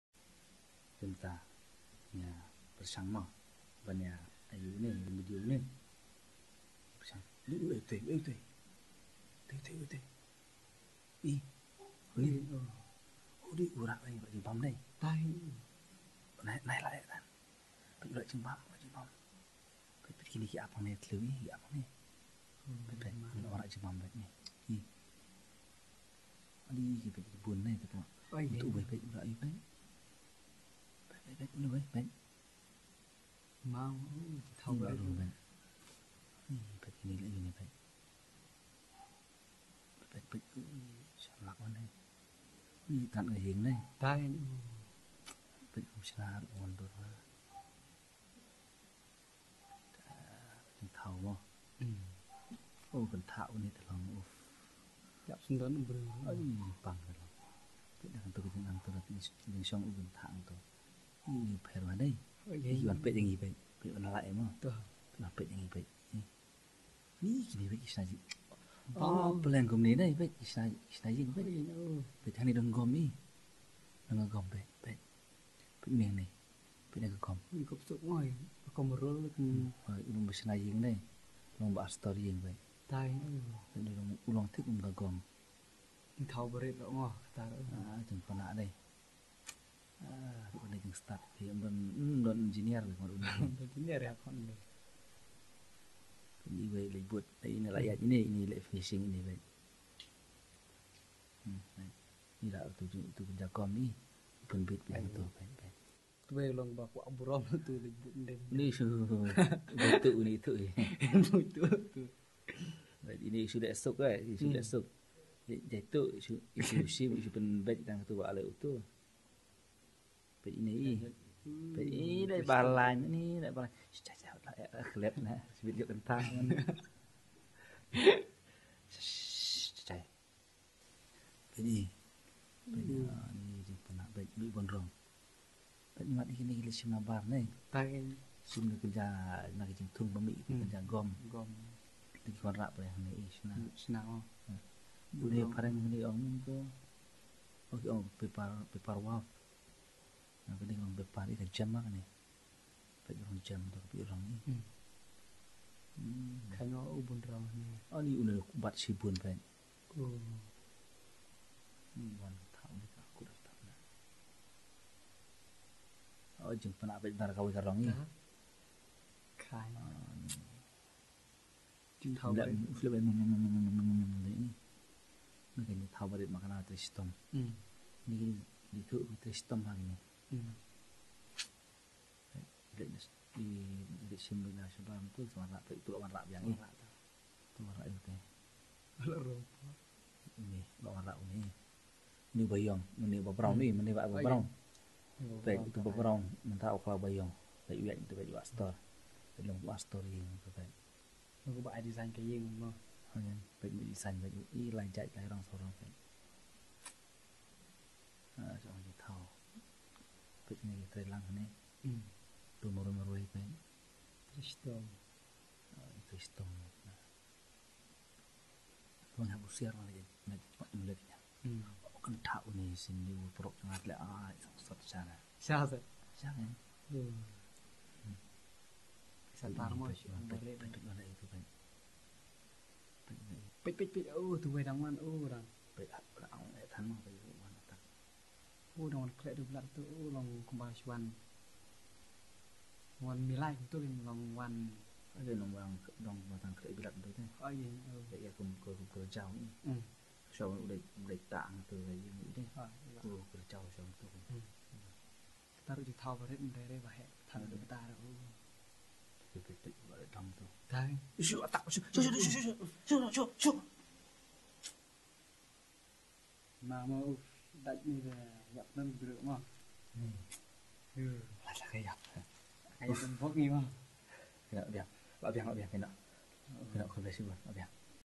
Amazing Architecture
Paper Wasps are the master builder,they are the small creatures but talented .